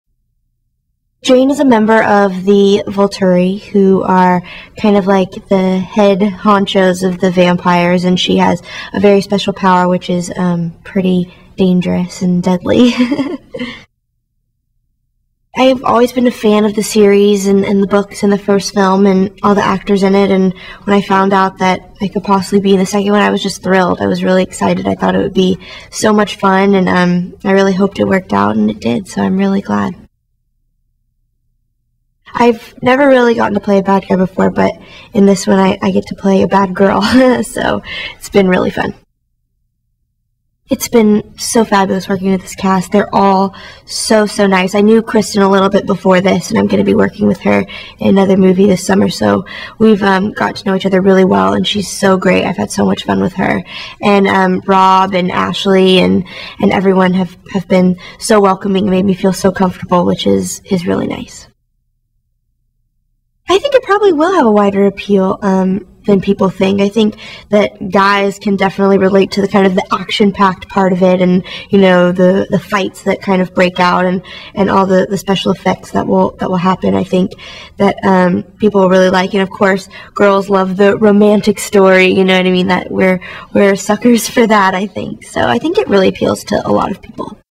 Dakota Fanning: "The Twilight Saga: New Moon" Interview
While I never post studio provided interviews, when you're dealing with a property like The Twilight Saga: New Moon, you make exceptions. Also, I know some of you Twilight fans and you want to see EVERYTHING.

Here's what you need to know: when actors are on set and filming a movie, at some point they do generic interviews for the eventual EPK (electronic press kit). Meaning some studio intern or production assistant asks the most basic questions and the actor answers them. They're not exactly hard hitting interviews. But, like I said, you Twilight fans want to see everything and I aim to please. So please enjoy the on set interview with Dakota Fanning